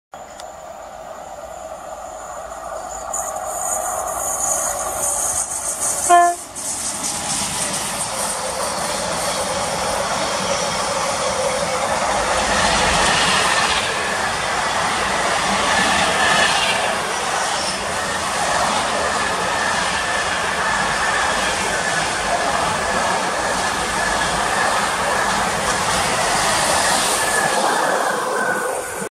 88003
88003 passes Thankerton with 4M48 the 14.52 Mossend Euroterminal to Daventry Tesco on Sunday 1 July 2018